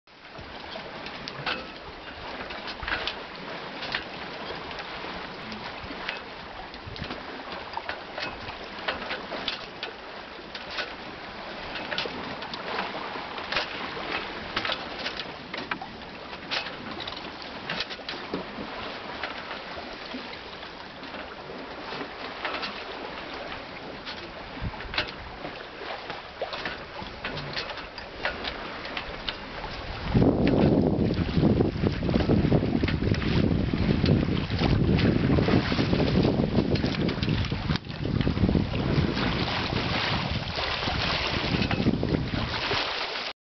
Zen Moment on Lake Michigan
Halfway across the lake with no land in sight, while my crew was off watch, the lake turned steely blue as the sun went down.